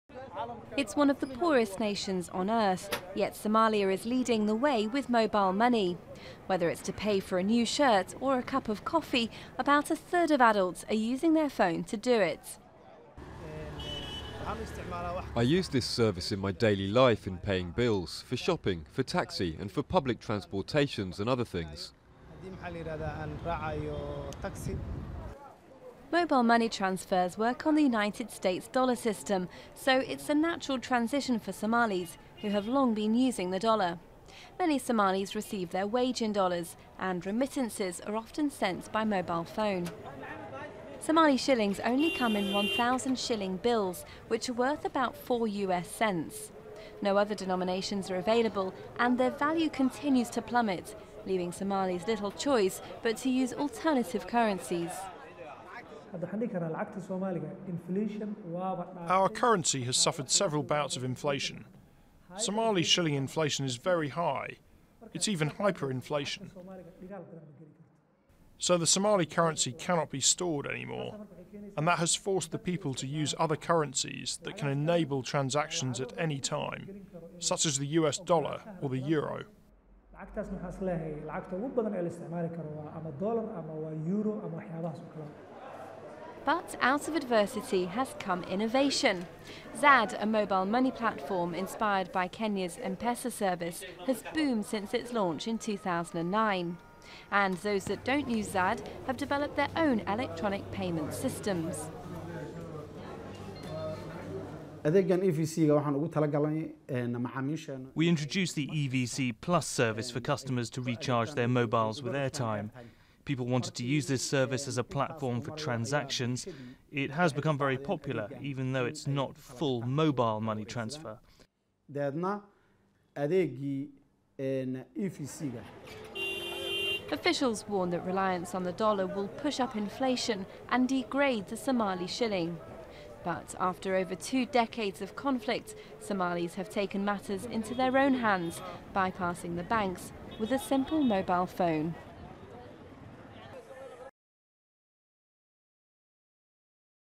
Somalia Abandons Shillings For Mobile Money Transfers
December 17, 2015 - With bank notes worth only 4 US cents each, Somalis have turned to mobile money to do their shopping, pay their bills and even receive their wages. It puts the East African country at the forefront of a financial revolution -- where the least developed countries in the world are leading the way.